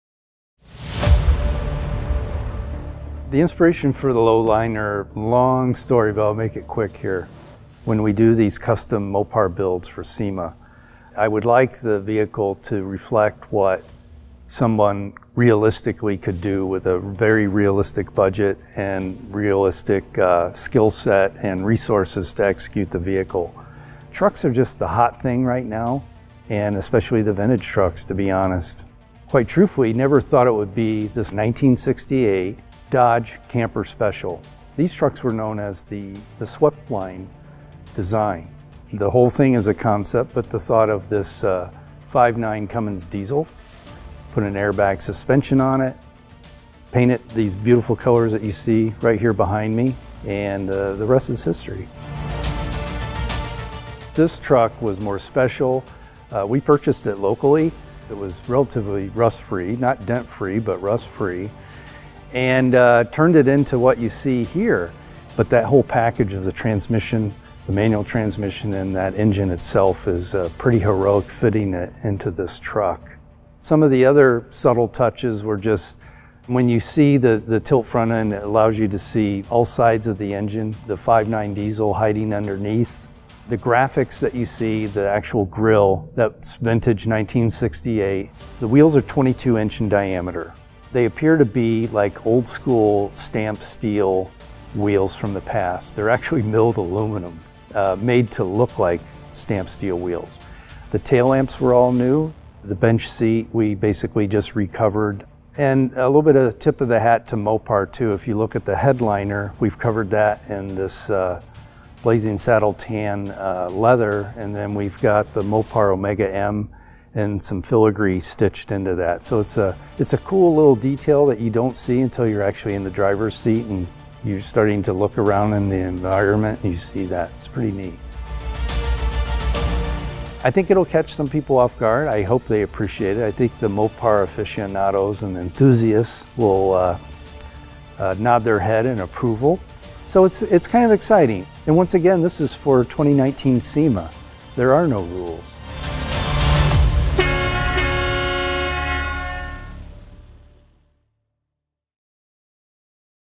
Mopar Lowliner - 2019 SEMA Feature
"Customization of classic trucks continues to be a hot trend in the aftermarket world, so Mopar designers rescued a 1968 Dodge D200-series heavy-duty pickup truck to transform it into the Candied Delmonico Red and Dairy Cream Mopar Lowliner Concept for this year’s SEMA Show.

Designers fully boxed the original C-channel frame of the rear-wheel-drive truck, substantially lowered the ride height and lengthened the wheelbase for a more dramatic stance. They also made a custom, power-tilt front end to showcase the legendary 5.9-liter Cummins diesel engine, which is mated to a six-speed manual transmission with a floor-mounted shifter."